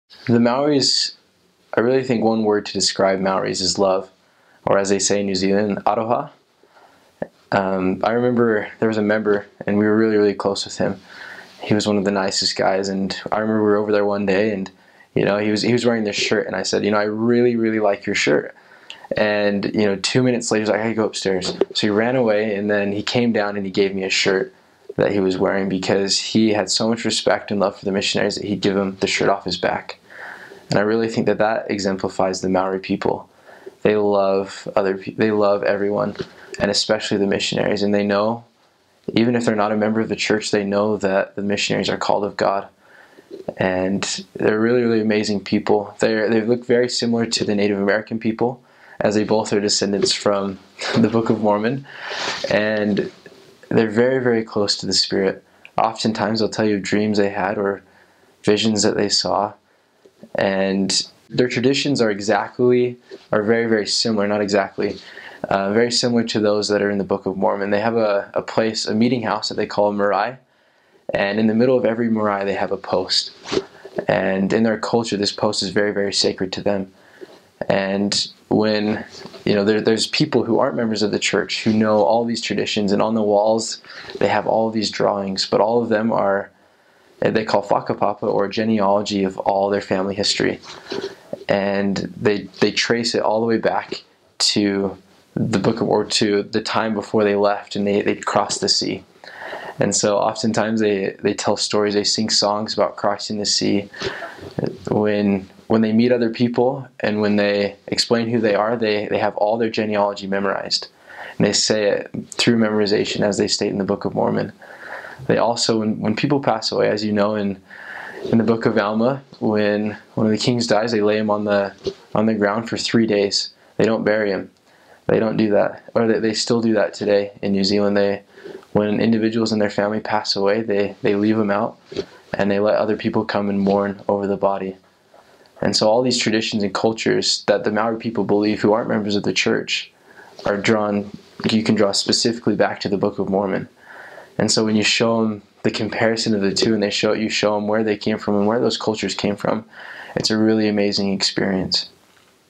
Maori People = Amazing
Maori People = Amazing. Cool facts about Maori people and culture (New Zealand).